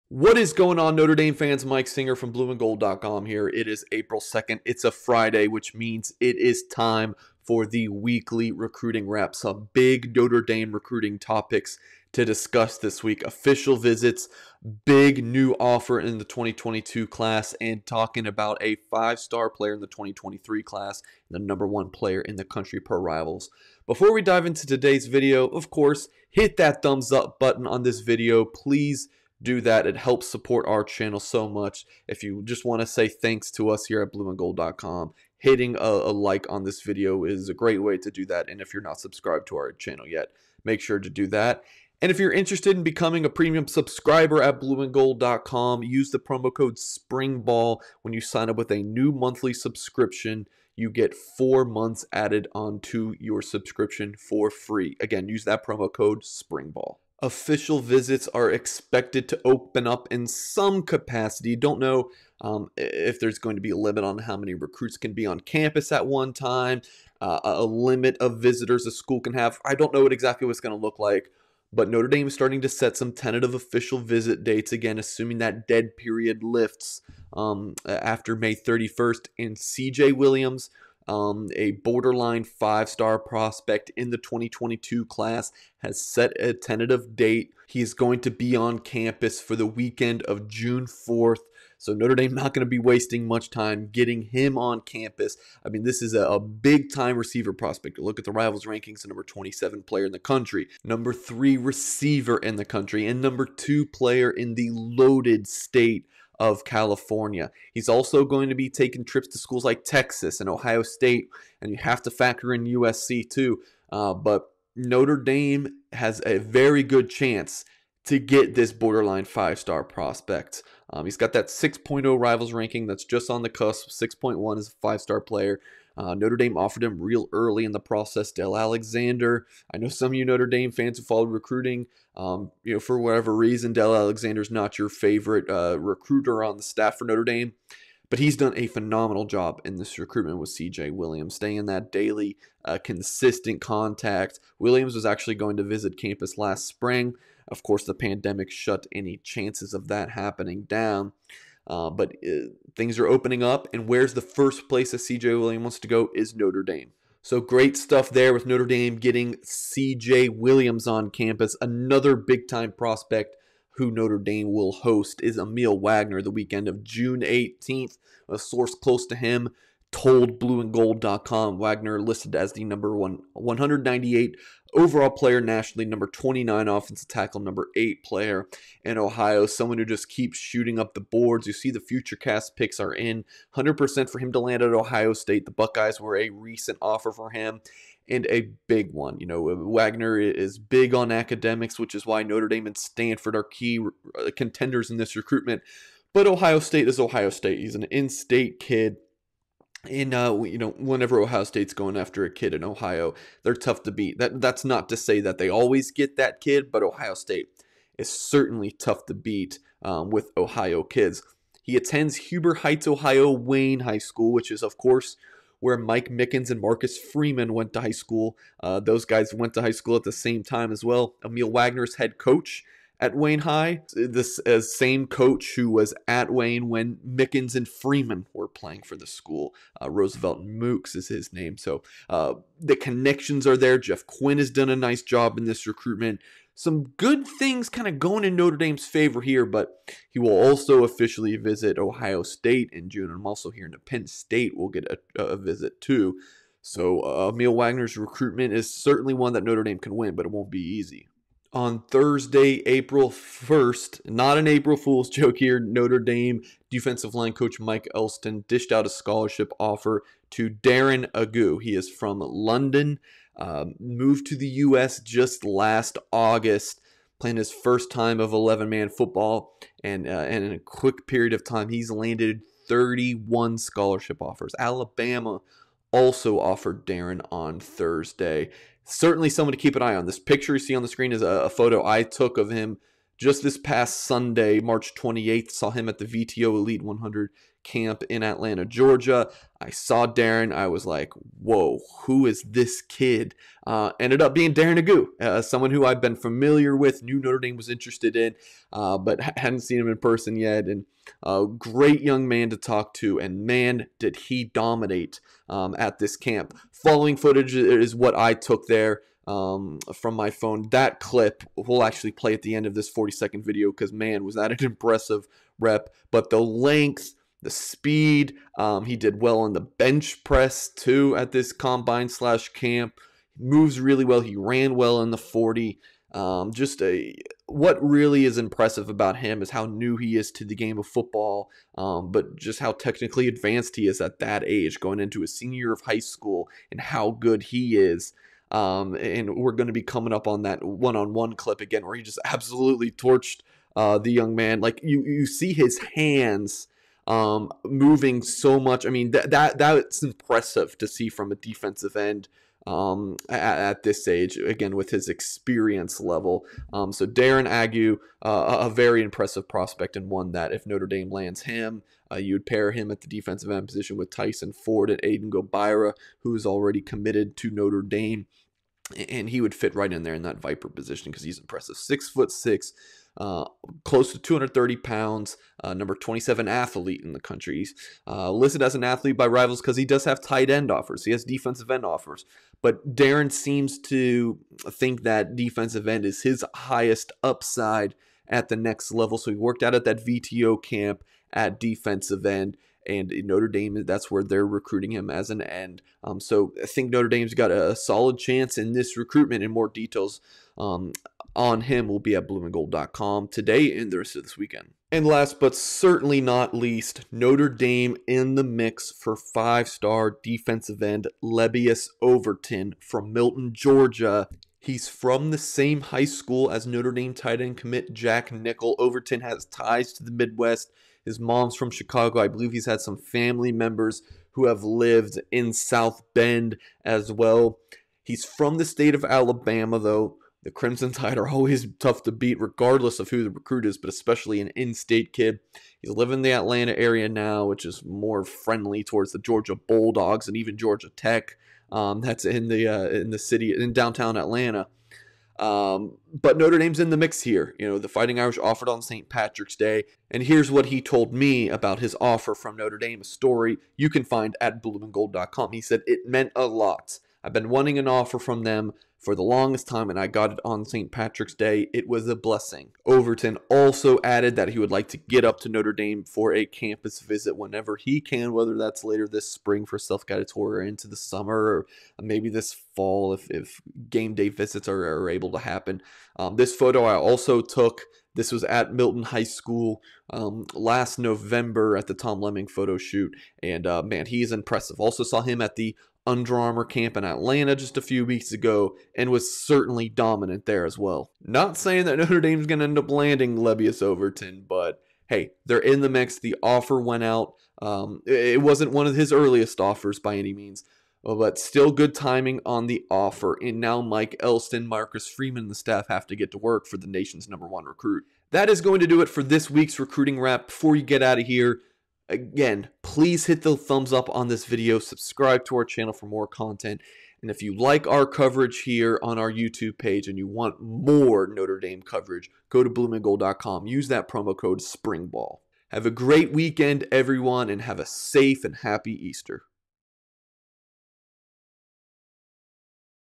Nation's No. 1 Recruit Speaks Highly Of Notre Dame | BIG-TIME Recruits Set Notre Dame Visits
BlueandGold.com recruiting insider Mike Singer is back with another episode of the Weekly Recruiting Wrap. This week, Singer discusses two big-time Notre Dame recruits setting official visit dates, an important new offer in the 2022 class and details on the Irish's pursuit of a five-star defensive end.